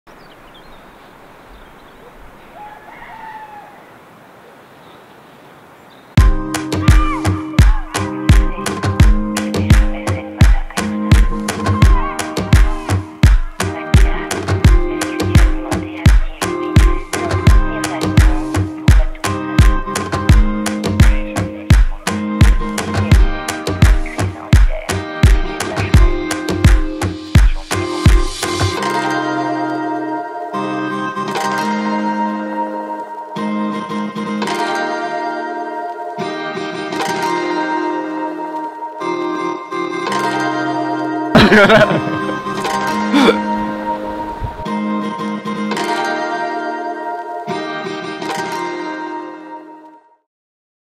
Teaser Week-end EB à Cantobre
Le team EB part en week-end. Cette année c'est Cantobre qui est pris d'assaut.

Follow the EB team during its annual team meeting. Cantobre, south of France, is the chosen one for 2016!